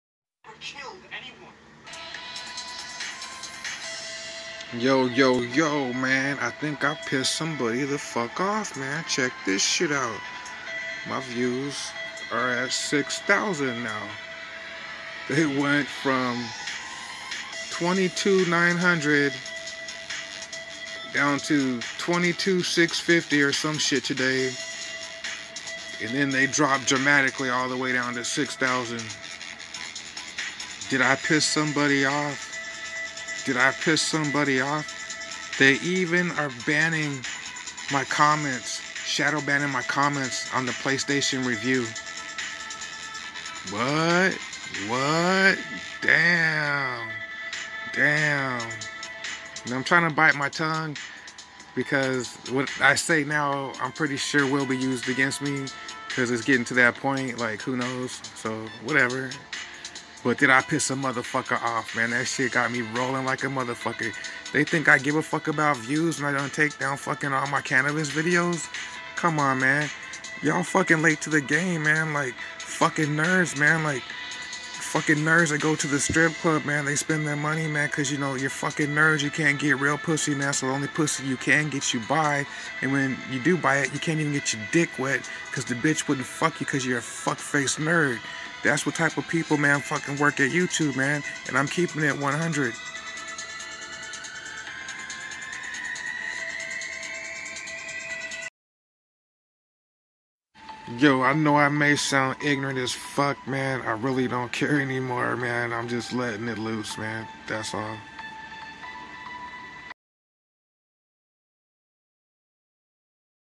Part2 showing 16,000 views deleted for speaking up about shadowbanning.
I dont give a fuck how I sound and I know I may sound stupid if your a squre.i cant sock a cunt in the face as in YouTube..could you imagine tho if I had a huge channel, spent mad money on channel to get treated like this.how I stable person could snap easily. And I mean a person who wouldn't hurt a fly type person..the quite nerdy ones are the ones not to fuck with.i gotta do this sponsor light grow so I will be doing a full grow on some platform..